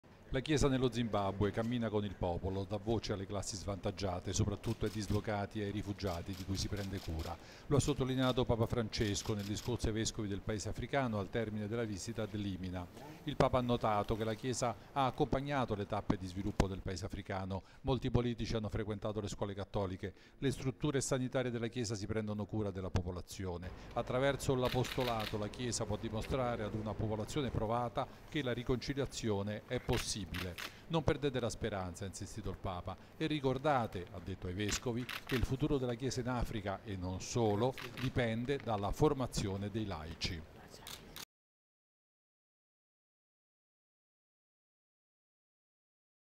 Papa Francesco vescovi Zimbabwe 02-06-2014
Unità e riconciliazione per lo Zimbabwe

Papa Francesco riceve in udienza in Vaticano i vescovi della Conferenza episcopale di Zimbabwe, in occasione della loro visita "ad Limina Apostolorum".